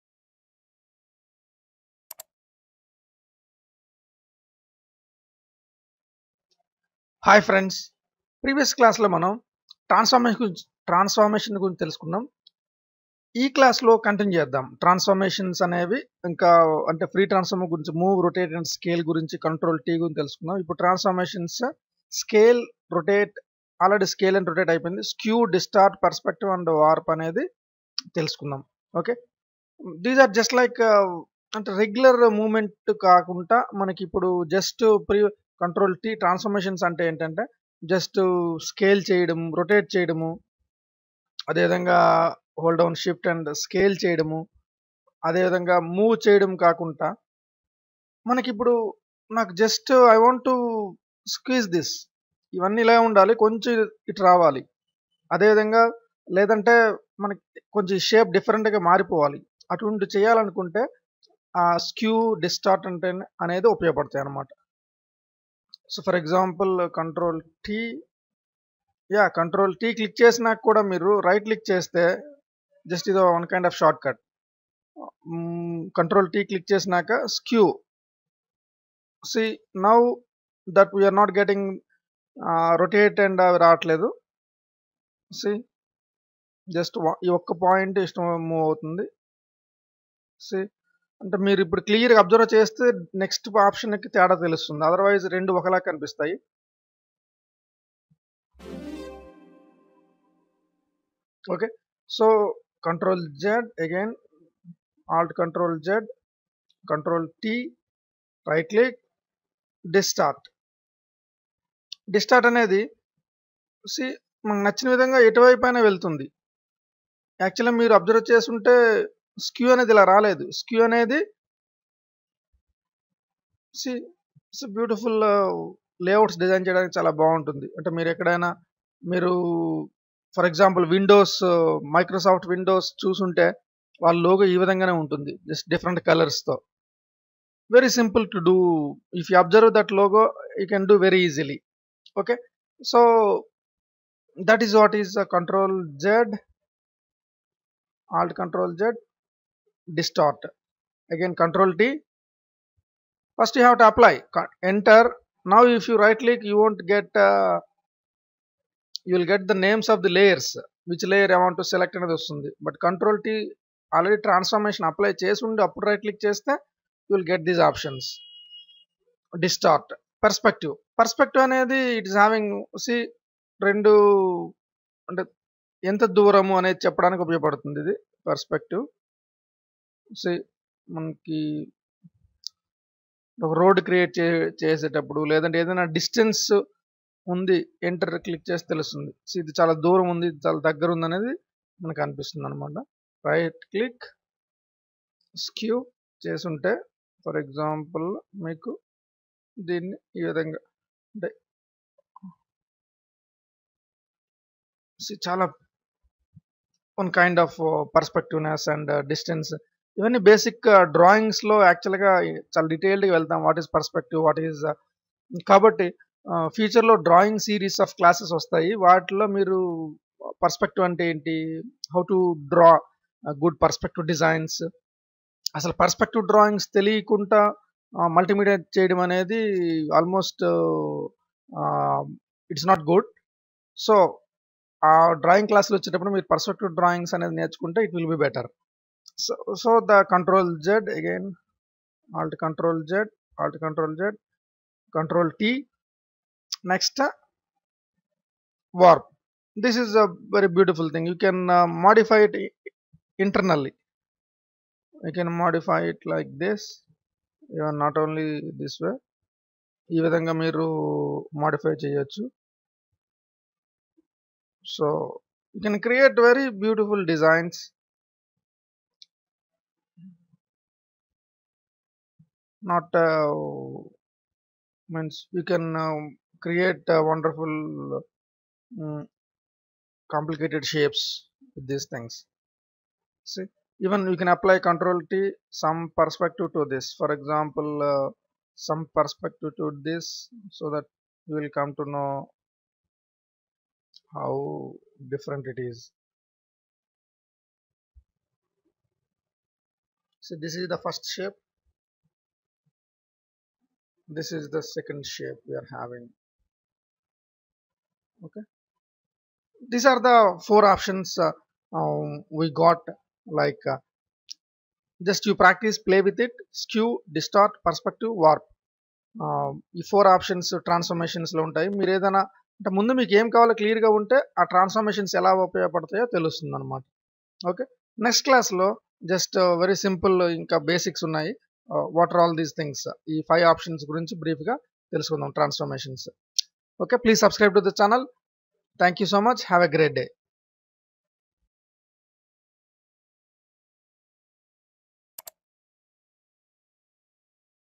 Learn Photoshop CS 6 - 16 - Transformations Skew Distort Perspective Warp (Telugu)
BEST For Multimedia, Photoshop & Image Editing Beginners.
Telugu Photoshop CS6 tutorials - taught by well experienced trainer.
Explained Transformations - Skew, Distort, Perspective & Warp Tools.
PLEASE COMMENT for any kind of specific tutorials - Ask in the comments.

for All Kinds of Telugu Videos.

- for all kinds of videos in English.

Happy Animation!